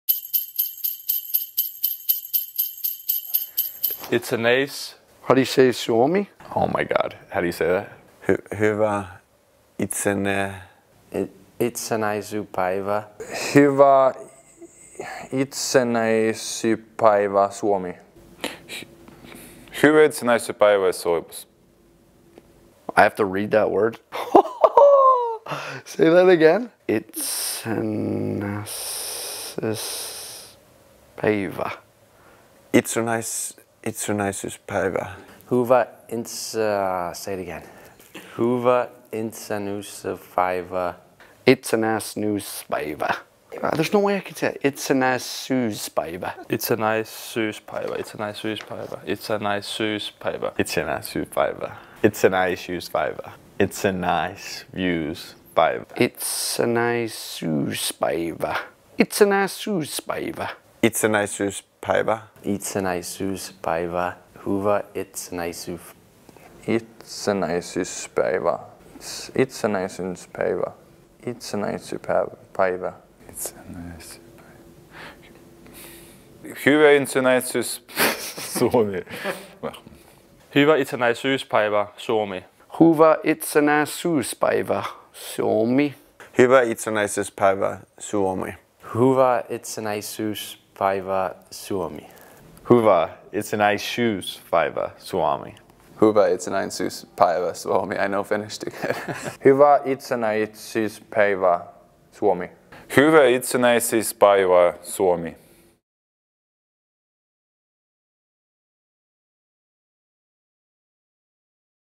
Joulukalenterin 6. luukku 2016
Joulukalenterin kuudennessa luukussa Jokereiden ulkomaalaispelaajat toivottavat Suomelle hyvää itsenäisyyspäivää - suomeksi.